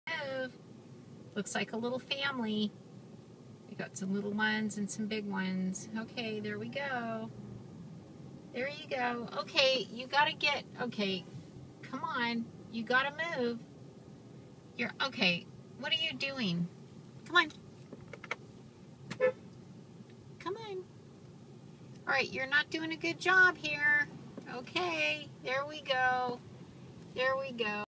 Turkeys
A little family on the road